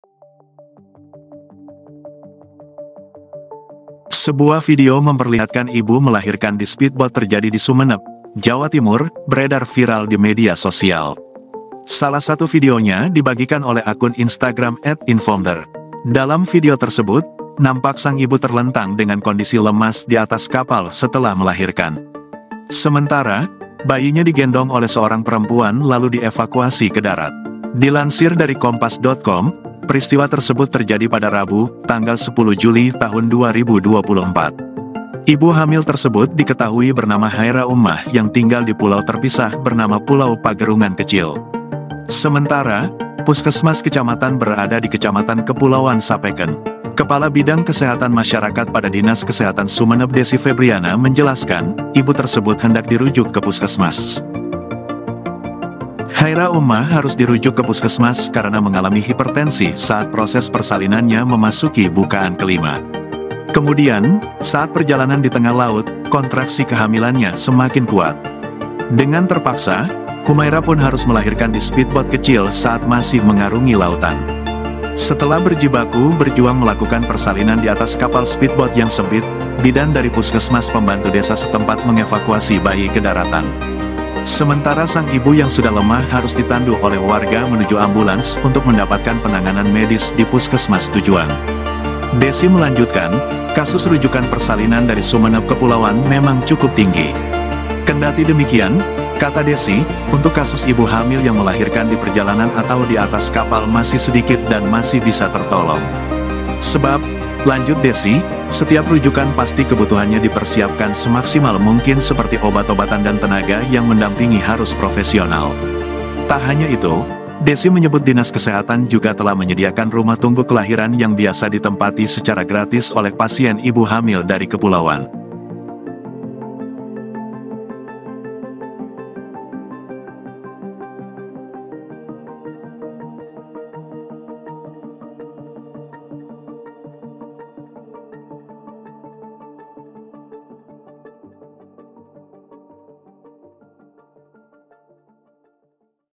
Viral Video Ibu Melahirkan Di Speedboat Di Sumenep, Tinggal Jauh Dari Puskesmas, Alami Hipertensi
Dalam video tersebut, nampak sang ibu terlentang dengan kondisi lemas di atas kapal setelah melahirkan. Sementara, bayinya digendong oleh seorang perempuan lalu dievakuasi ke darat.